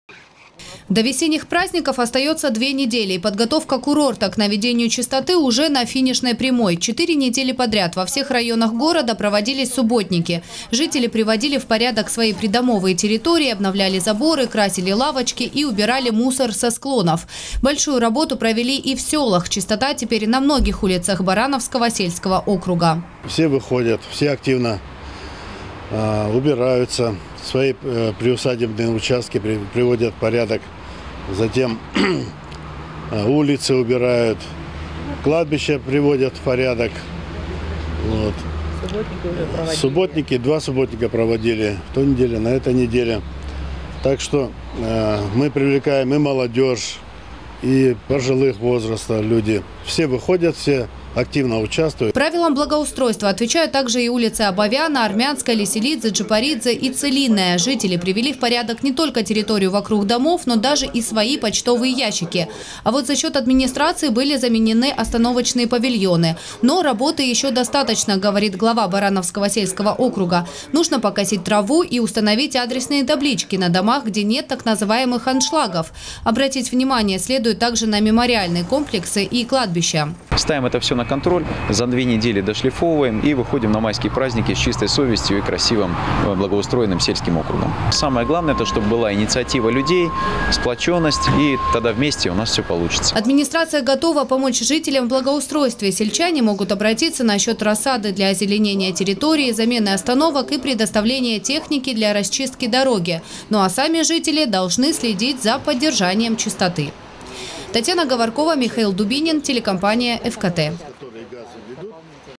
Жителей сочинских сел призвали к чистоте. Новости Сочи Эфкате
Во всех районах курорта, по поручению главы города Анатолия Пахомова, проводятся субботники. Благоустройство и приведение к надлежащему санитарному состоянию касается и сел. Накануне в администрации Барановского сельского округа прошло совещание с представителями общественности и районными депутатами.
Сочи. Новости.
Телеканал Эфкате.